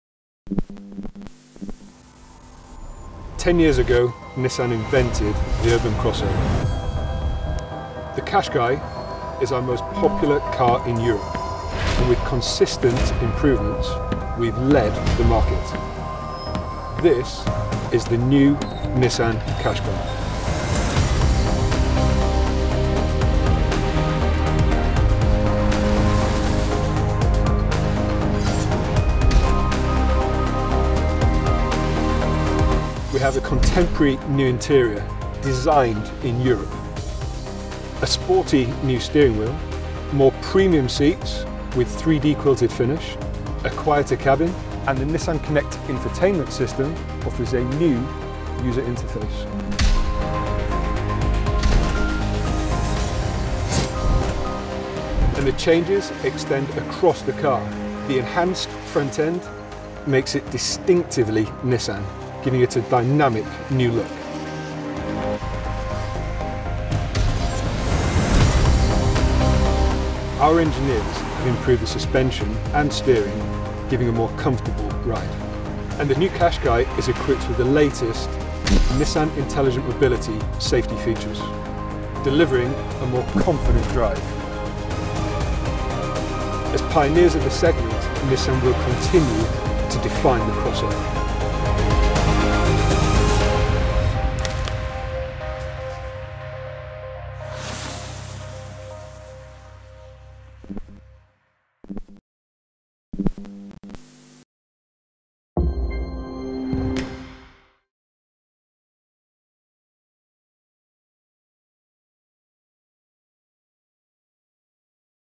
2017 Nissan Qashqai facelift product insight
The Qashqai has always been known for its confident drive. Thanks to a number of under-the-skin improvements, the new model maintains its outstanding handling performance. Modifications to the suspension, damping and steering systems have resulted in an even more refined on-the-road experience.

Improved use of absorption materials and increased rear glass thickness have led to a reduction in cabin noise from the road, engine and wind. Striking new 17, 18 and 19-inch alloy wheels are available, all aerodynamically optimised to maximise efficiency.

Even with so many enhancements, the Qashqai’s aerodynamic performance has not been compromised. It retains its class-leading drag coefficient of 0.31, and continues as segment leader on C02 emissions, with only 99g/km from the 1.5-litre dCi 110 diesel. Such innovation contributes to low running costs for Qashqai drivers.